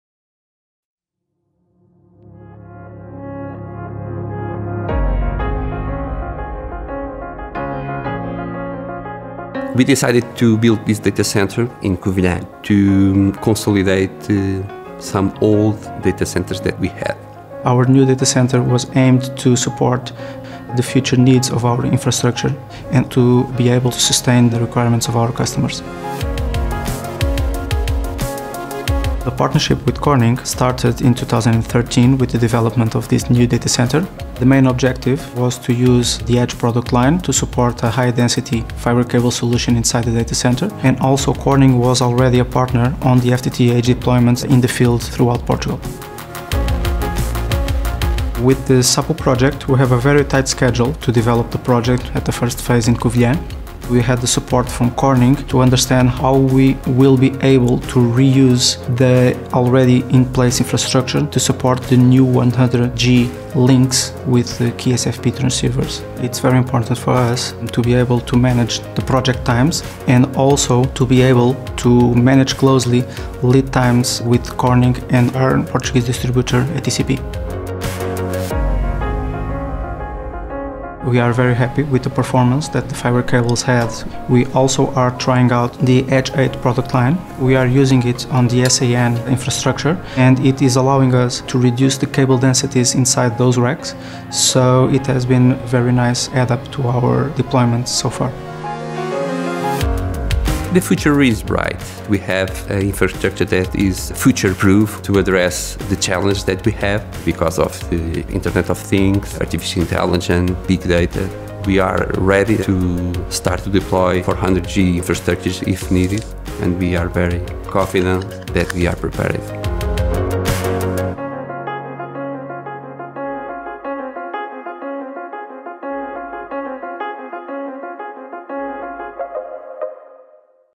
Serviços de dados e comunicações preparados para o futuro
Um case-study Altice elaborado pela Corning.